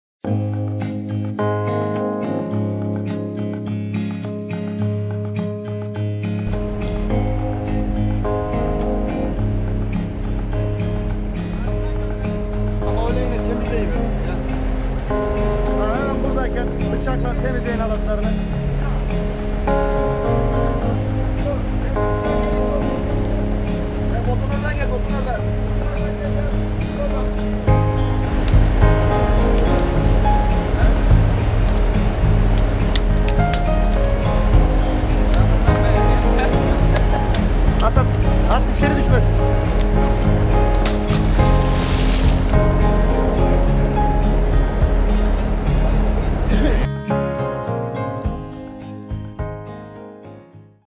Deniz kaplumbağasını balıkçılar kurtardı
Akdeniz açıklarında ağ ipine dolanan deniz kaplumbağasını balıkçılar kurtardı.